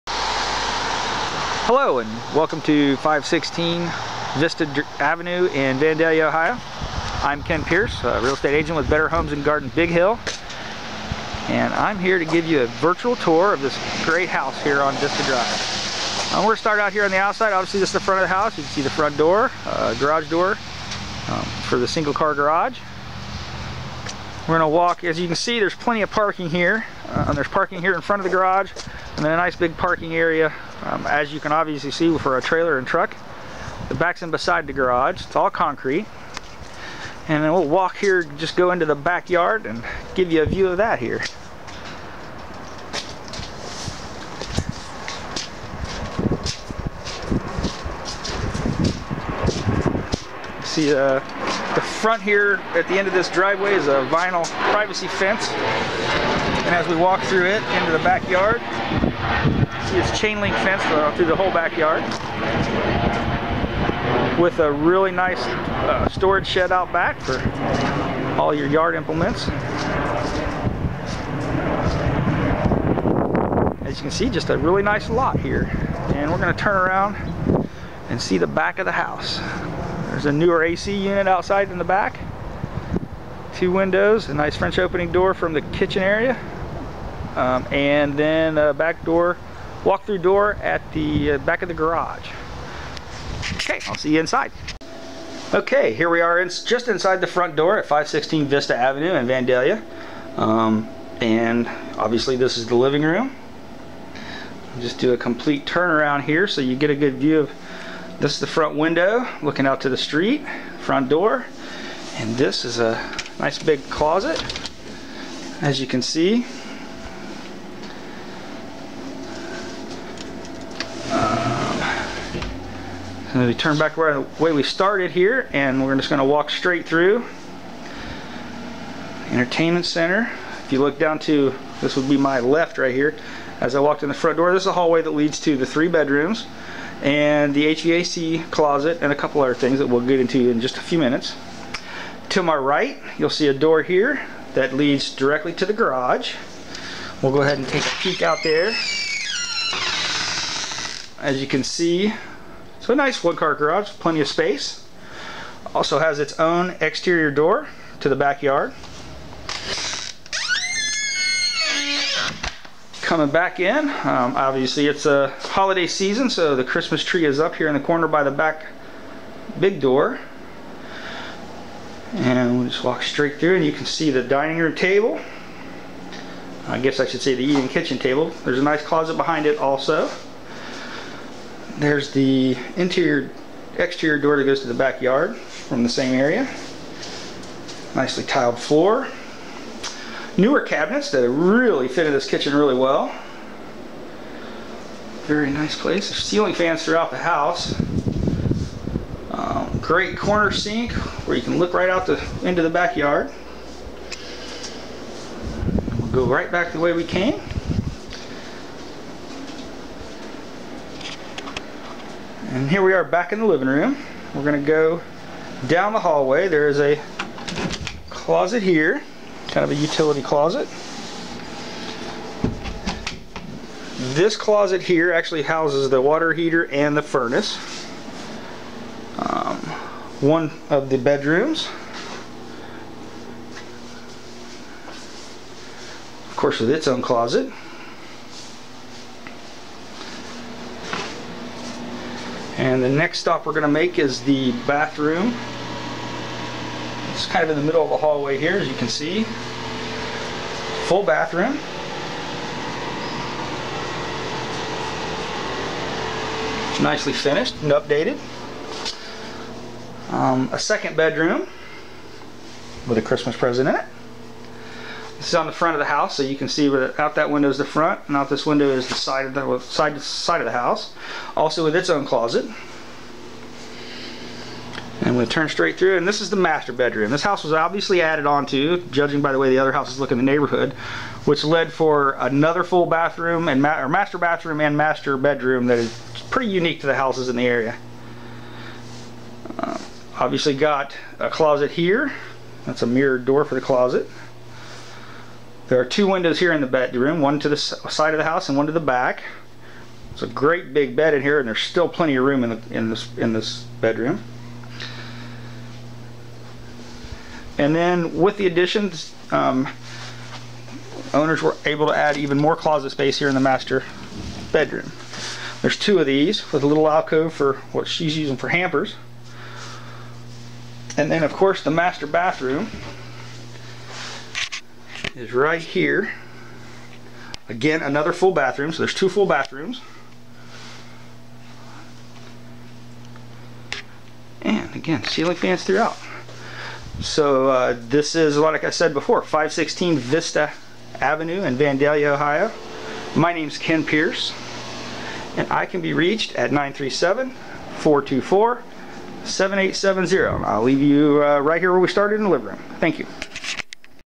SOLD!!! Sparkling Clean 3BR, 2BA Home in Vandalia. This house has an updated kitchen & master suite.
516 Vista Ave, Vandalia, OH 45377 $109,900 This is the one you have been waiting for! A sparkling clean, well-cared for brick and vinyl ranch that is move-in ready!  You will be impressed from the moment you pull up and see the deep garage and extended driveway that can accommodate a large truck and trailer.  Walk through the gorgeous front door and you will be welcomed by the spacious living room that leads to the updated kitchen.  This home is truly unique to the neighborhood, as it was updated to offer a larger kitchen and create a master suite with three closets and a full bathroom!  Additional updates include new furnace, A/C, range and dishwasher in 2010, as well as a new refrigerator in 2013!   The large backyard is completely fenced and offers a huge storage shed.  This home won't last long so schedule your private appointment today!
Welcome to Vandalia, OH
Welcome to Vandalia-Butler City Schools
Vandalia-Butler Elementary Schools
Vandalia-Butler Middle Schools
Vandalia-Butler High Schools